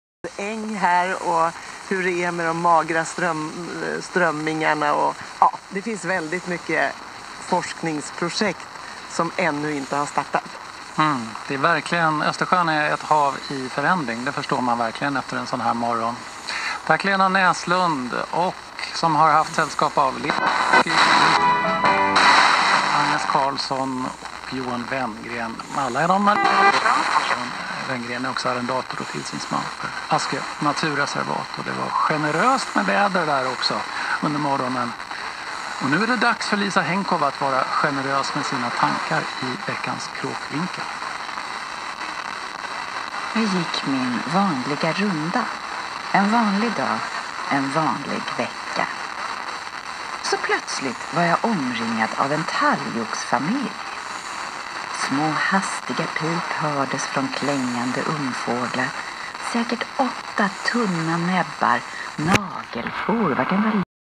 [Tropo]89.3 h SR P1(Sollefteå/Multrå-Flugtjärnshöjden)(Sweden)(Västernorrland)~702km 245 m 60 kW(ERP
receiver:Degen 1103
antenne:BUILT-IN TELESCOPIC(93 cm)
location:Priozersk(SPb-Region)
time 09:28 GMT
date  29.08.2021

station accepted for the first time

Compared with:88.5 h SR P1(Vännäs/Granlundsberget)(Sweden)(Västerbotten)~619km 267 m 60 kW(ERP)

Приём на местности с высотой 44 м над уровнем моря по топографическим картам

Reception in an area with a height of 44 m above sea level according to topographic maps